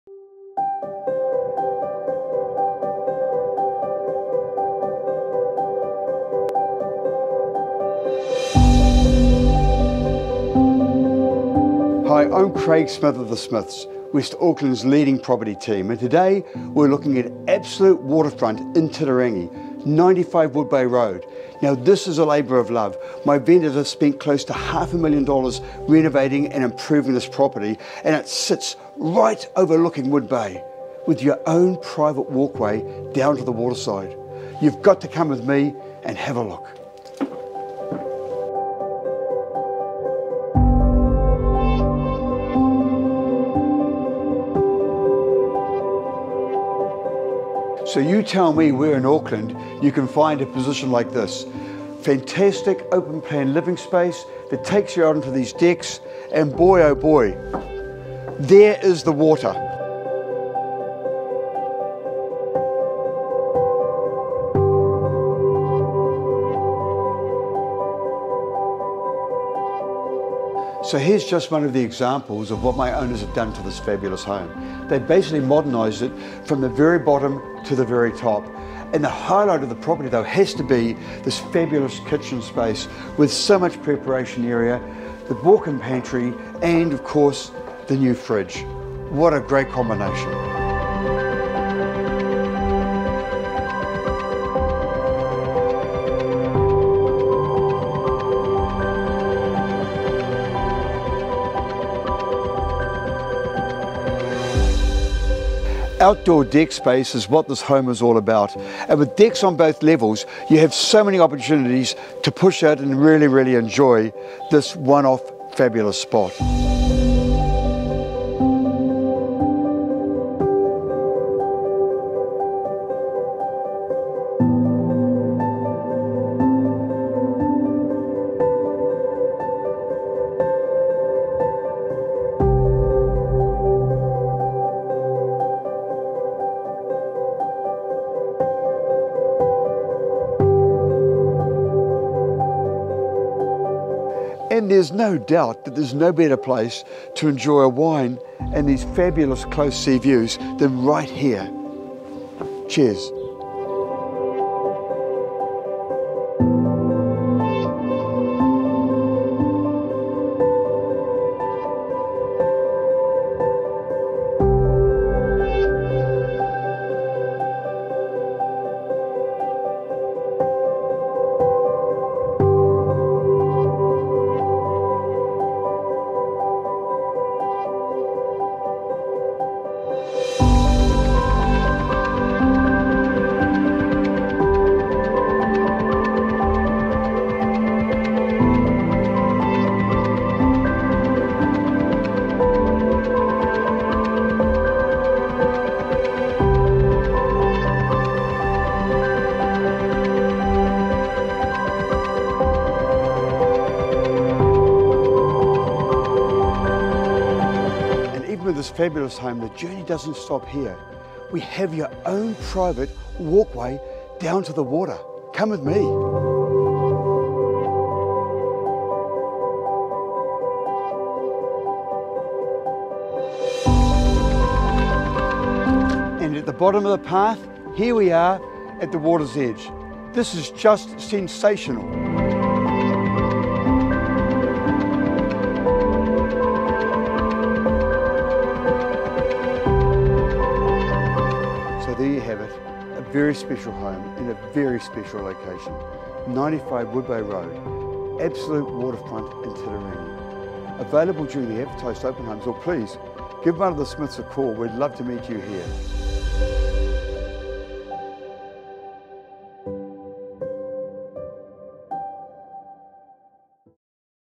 95 Wood Bay Road, Titirangi - The Smiths - Ray White Austar Group
Seaside Sanctuary

Absolute waterfront living is on offer here with this beautifully modernized home in the sought after Titirangi Bays region. Sitting proudly on a modest headland this property has close and intimate views looking over Wood Bay Beach. Seaside environments offer a unique and stunning environment, from the smell of fresh salt air to the restful lapping of waves against the seashore. Here we have a perfect design, with open plan living delivering easy access to spacious decks, so ideal to drink these incredible views in from. This has been a labour of love for our vendors that have literally redesigned and refurbished this home from top to bottom so that the next owner can enjoy the new stylish bathrooms, modern décor and fabulous kitchen, the focal point of the home. There’s even a bit of grass to enjoy with a hidden pathway that takes you down to the water. This is a truly special home in an ultra special seafront location, unable to be repeated so not to be missed.